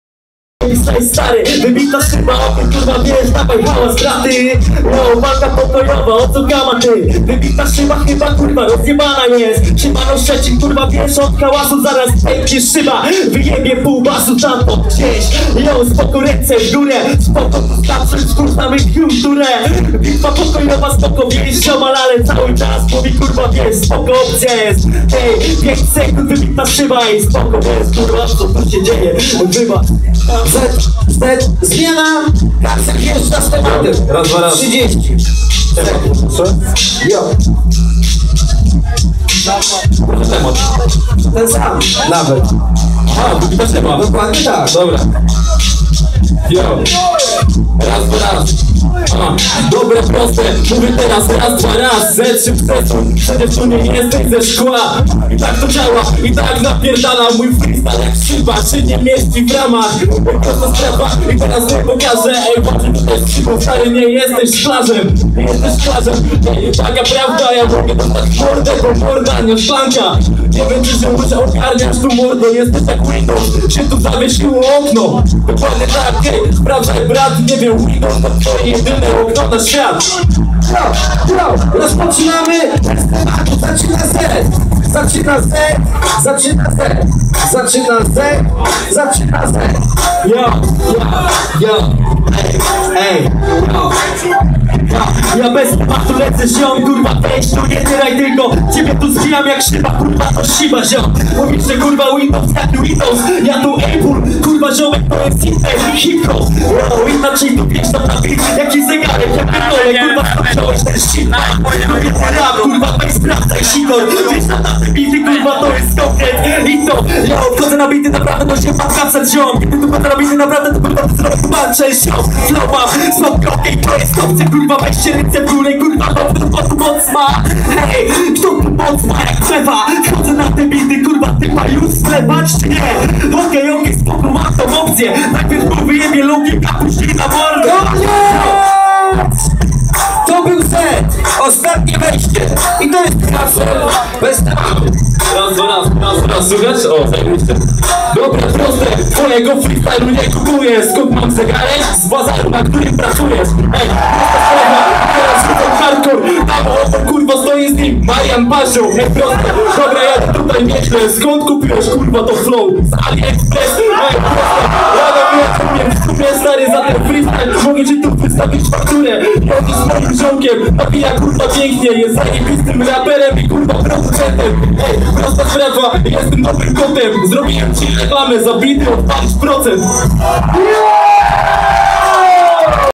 Kapsel vs Zet (WBS vol. 10) 2016
Kapsel vs Zet (1/8 WBS)

Wielka Bitwa Szczecińska - edycja nr 10.
Klub: Szczeciński loft kultury (ex. Wasabi).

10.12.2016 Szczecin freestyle

Nagranie: Piotr Hołowko